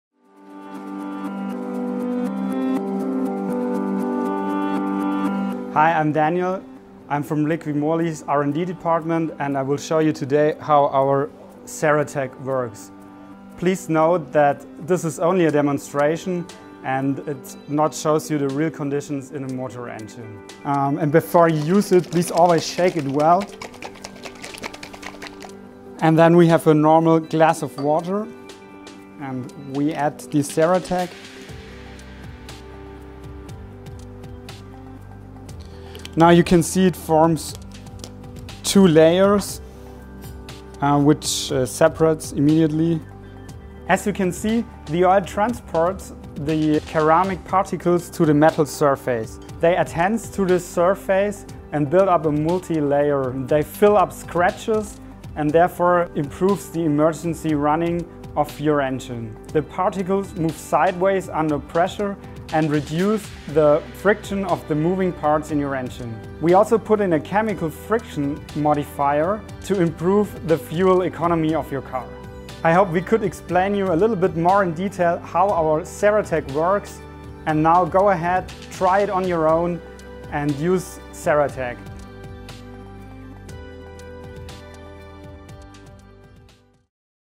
Cera Tec - Subtítulos en español
Cera Tec es un aditivo de alta tecnología con partículas cerámicas para proteger el motor contra el desgaste. Reduce la fricción y el desgaste a través de compuestos cerámicos, ofreciendo una alta resistencia química y térmica. Evita el contacto directo del metal con el metal, lo que aumenta la vida útil del motor. Reduce el consumo de combustible en motores de gasolina y diesel, gracias al efecto de baja fricción. Miscible con todos los aceites de motor disponibles comercialmente y aceites para engranajes de vehículos de motor. Vea en una simulación cómo reacciona el aditivo con el metal, protegiéndolo.